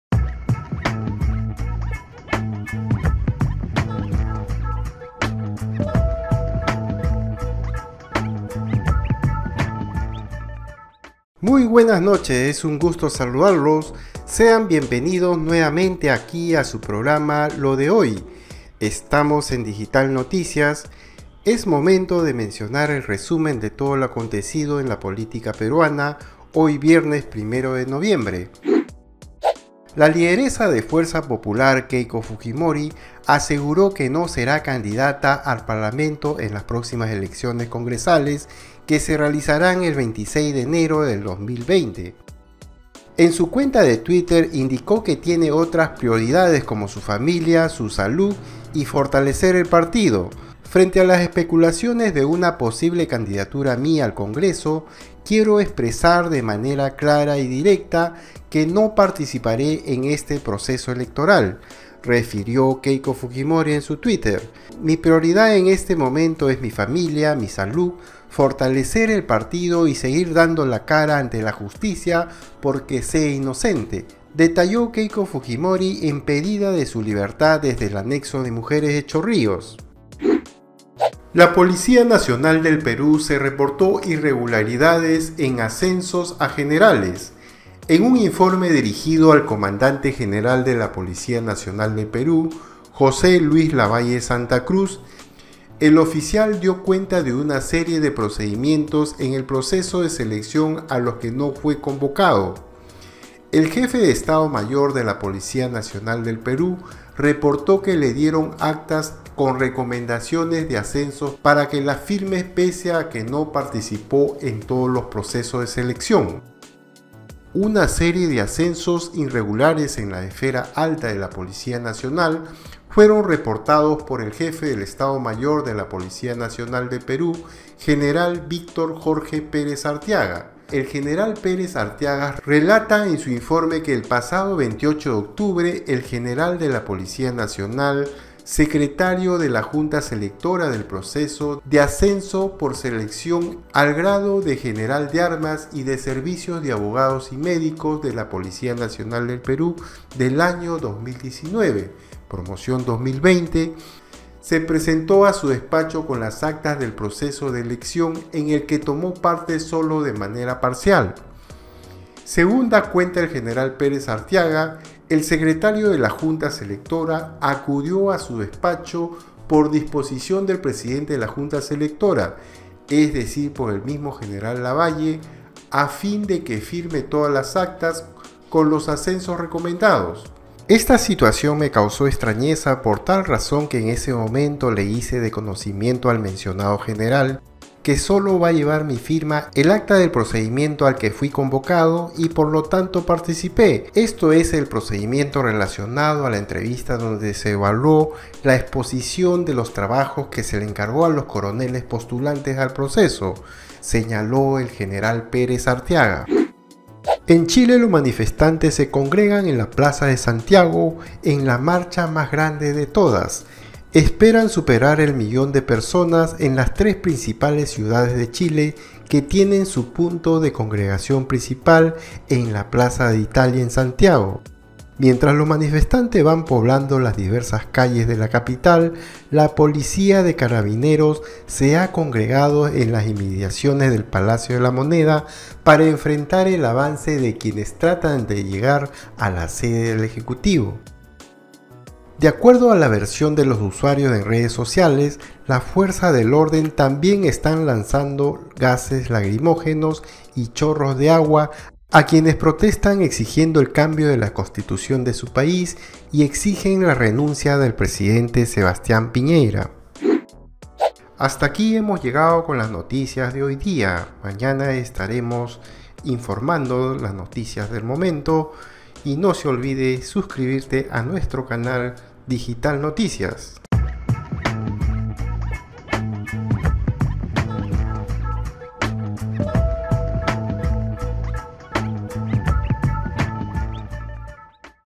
LO DE HOY -Keiko Fujimori DESCARTAR POSTULAR al CONGRESO en el 2020
LO DE HOY -

Se reporto irregularidades en ascensos a generales.

En Chile: manifestantes se congregan en Plaza de Santiago en la marcha más grande de todas.

Nosotros:
Web: